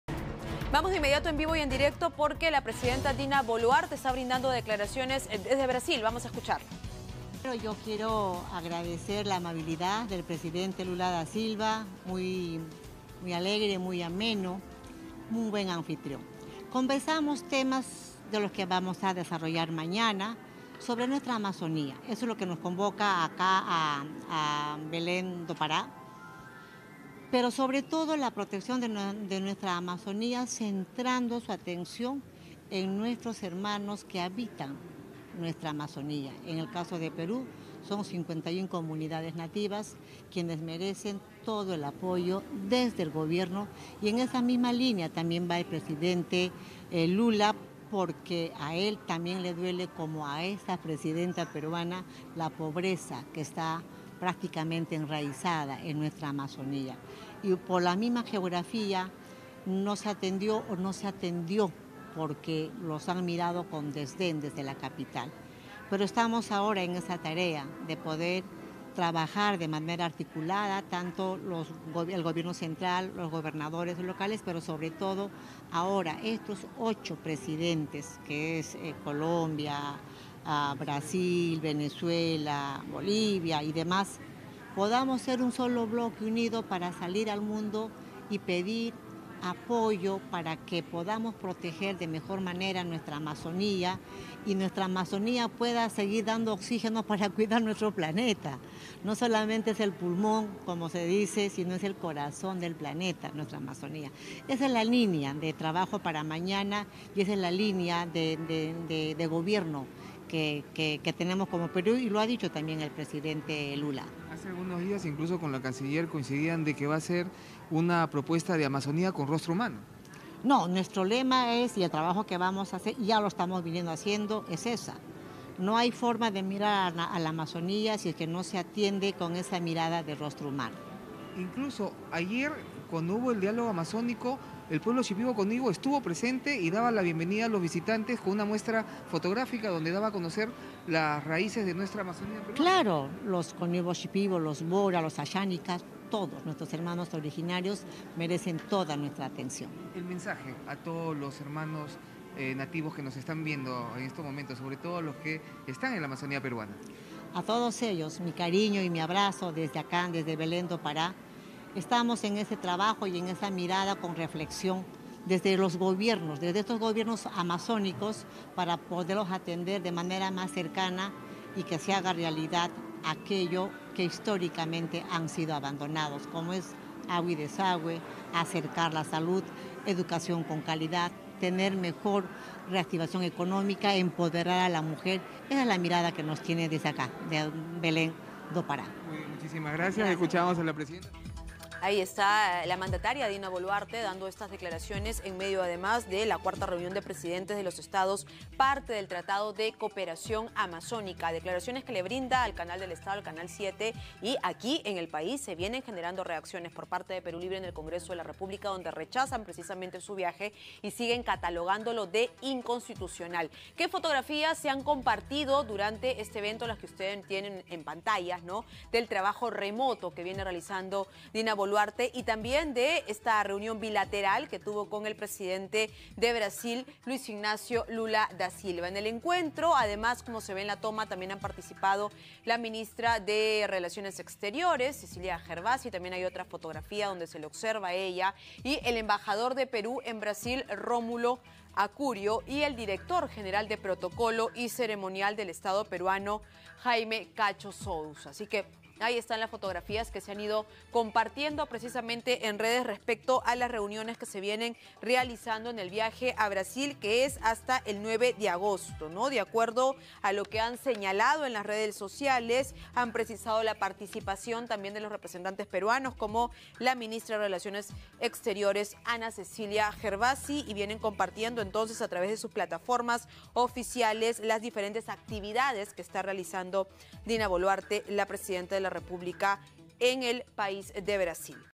Dina Boluarte tras reunión con Lula da Silva: "Trabajaremos en la protección de nuestra Amazonía"
La mandataria brindó declaraciones en el marco de su participación de la reunión de los Estados del Tratado de Cooperación Amazónica.